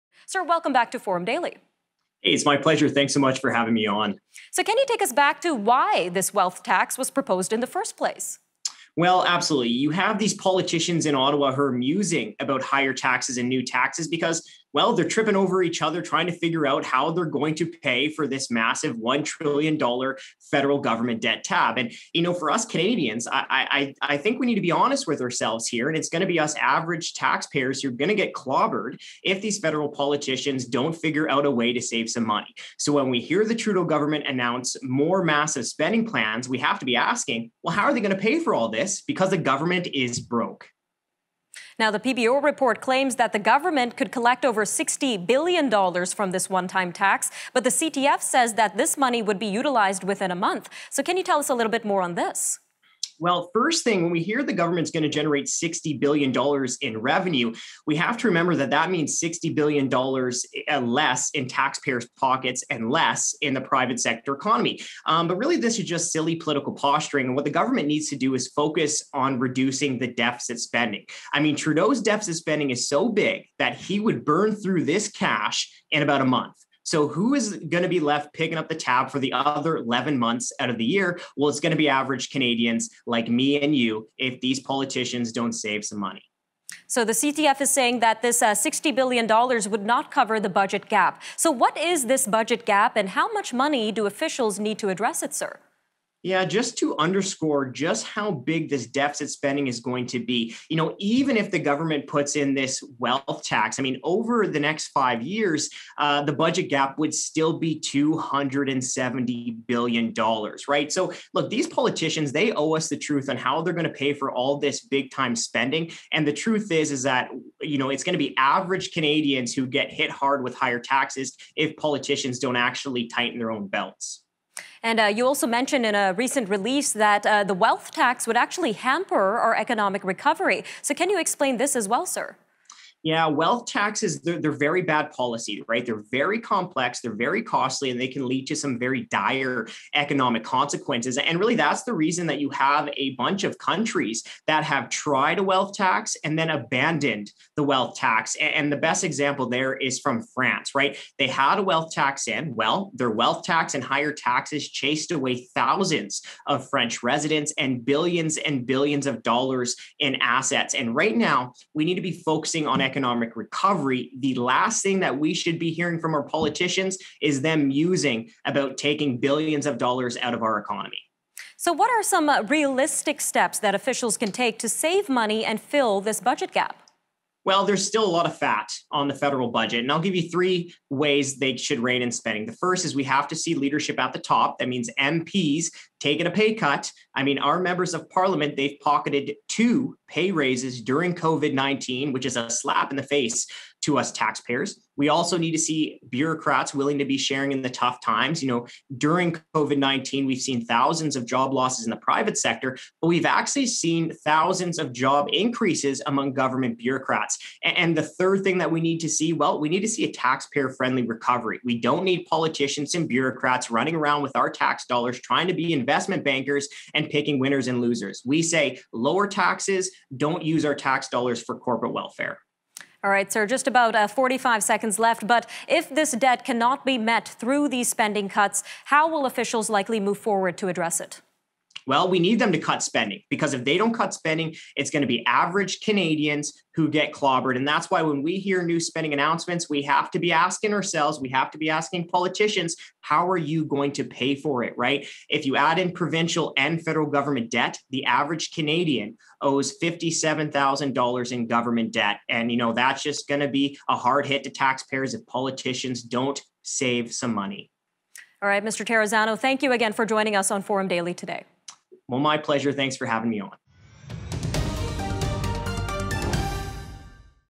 CTF: Liberals Will Spend Wealth Tax In a Month - Needs a Better Approach for Budget Gap –Segment
Watch The News Forum for more news, updates, and in-depth interviews from a Canadian conservative-friendly perspective tonight on Bell, Telus, and Access Communications.